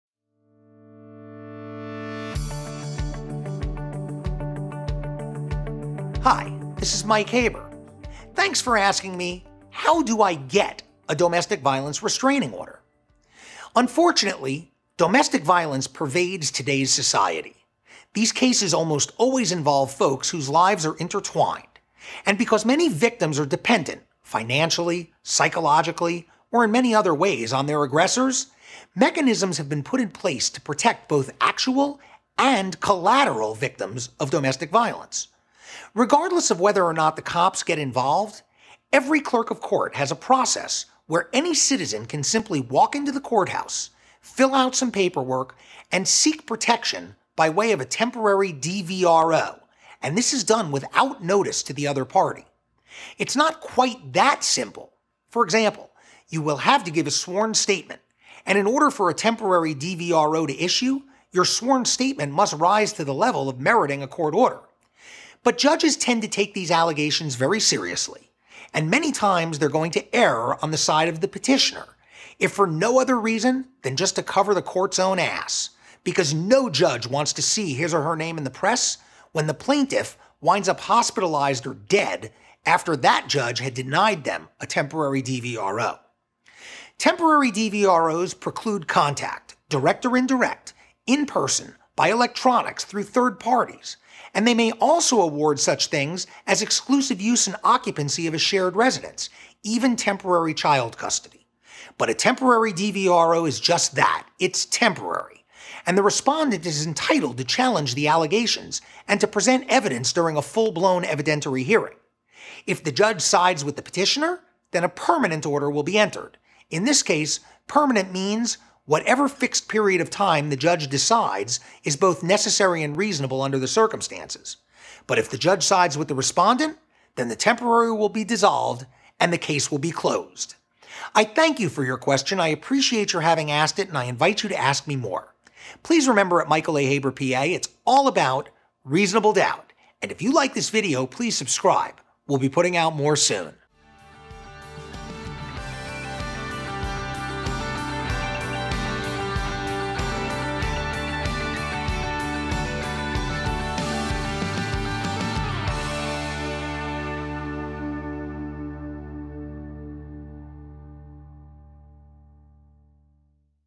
210) How do I get a Domestic Violence Restraining Order? ~ Michael A Haber Miami Criminal Lawyer
Thank you for asking me “How do I get a Domestic Violence Restraining Order/ DVRO?”

Unfortunately domestic violence pervades today’s society.  These cases almost always involve folks who’s lives are intertwined, and because many victims are dependent - financially, psychologically or in many other ways - on their aggressors, mechanisms need to be in place to protect both actual and collateral victims of domestic violence.

Regardless of whether or not the cops are involved, every Clerk of Court has a process where a citizen can simply walk in to the courthouse, fill out some paperwork and seek a temporary DVRO without notice to the other party.

It’s not quite that simple, for example, you have to give a sworn statement, and in order for a temporary the DVRO to issue the sworn statement must rise to the level of meriting a Court Order, but Judge’s tend to take these allegations very seriously, and many times they err on the side of the Petitoner, if for no other reason than to cover the Court’s own ass - because no Judge wants to see her or his name in the press when the Plaintiff winds up hospitalized or dead after that Judge had denied them a temporary DVRO.

A temporary DVRO precludes any contact, direct or indirect, in person, by electronics or through third parties, and it may also award such things as exclusive use and occupancy of a shared residence and even temporary child custody.

But the temporary DVRO is just that, temporary, and the Respondent is entitled to challenge the allegations and to present evidence during a full blown evidentiary hearing.  If the Judge sides with the Petitioner then a permanent Order will be entered (“permanent” meaning for whatever fixed time period that the Judge decides is necessary and reasonable in that case); and if the Judge sides with the Respondent then the temporary will be dissolved and the case closed.